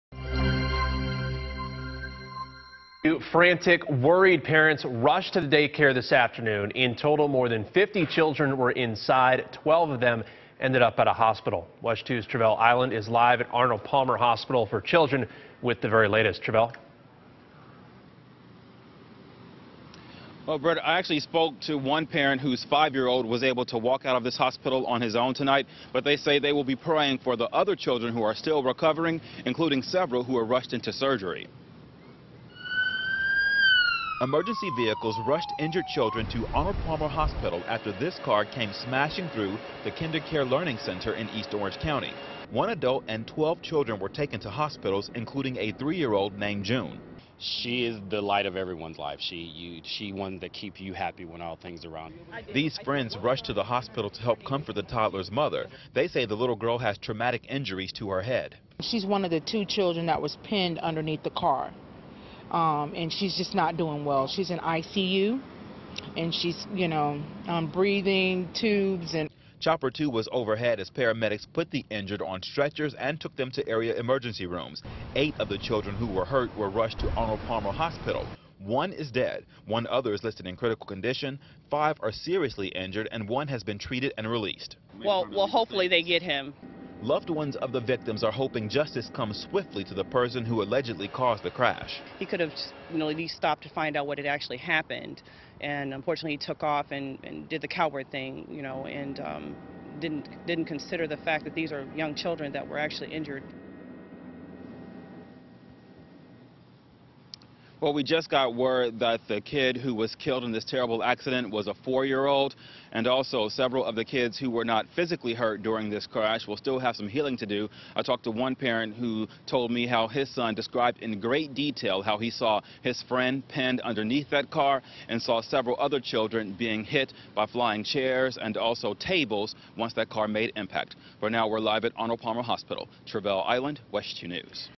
Frantic, worried parents rush to day care, hospital following hit-and-run crash
One person has died and 14 others were injured when an SUV ran a vehicle off the road and into an Orange County day care Wednesday afternoon.